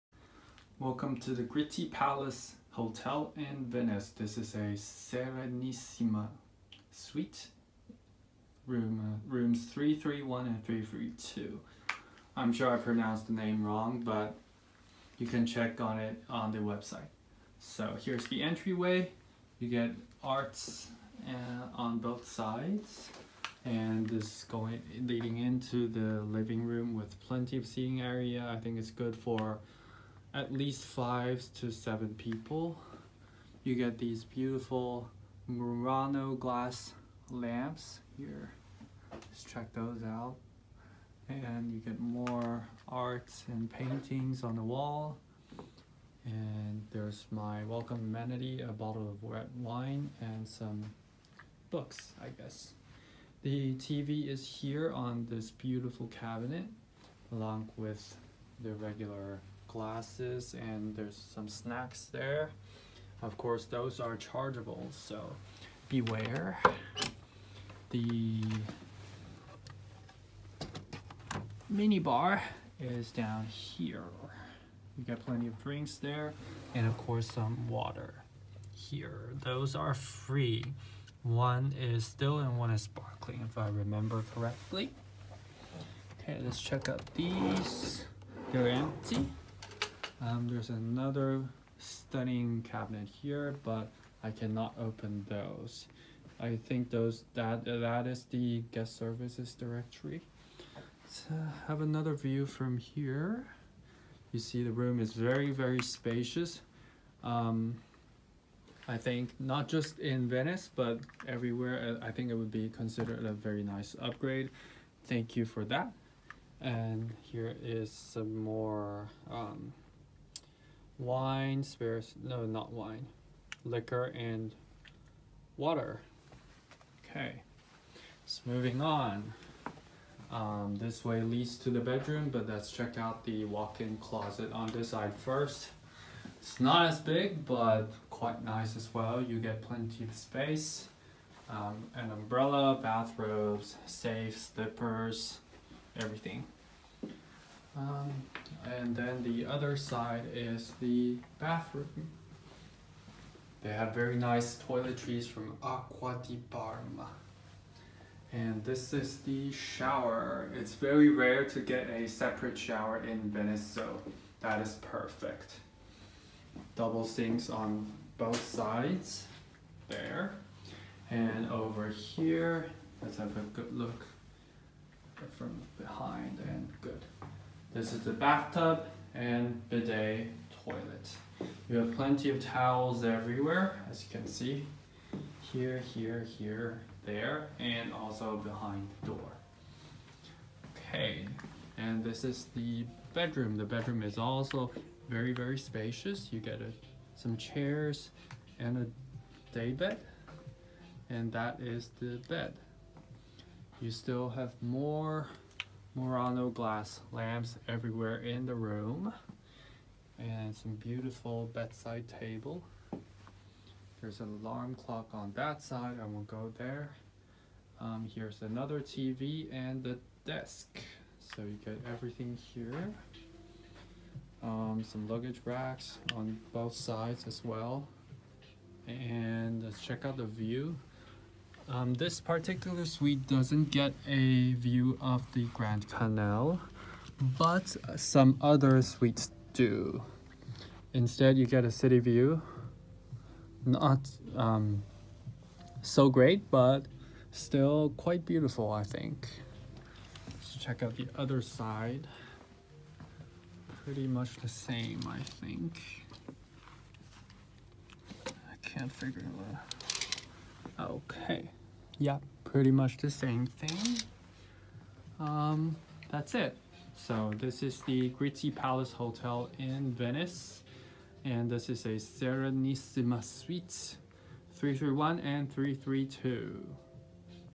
The Gritti Palace, a Luxury Collection Hotel, Venice - Serenissima Suite 331/332
Review of Serenissima Suite 331/332 at the Gritti Palace, a Luxury Collection Hotel, Venice, Italy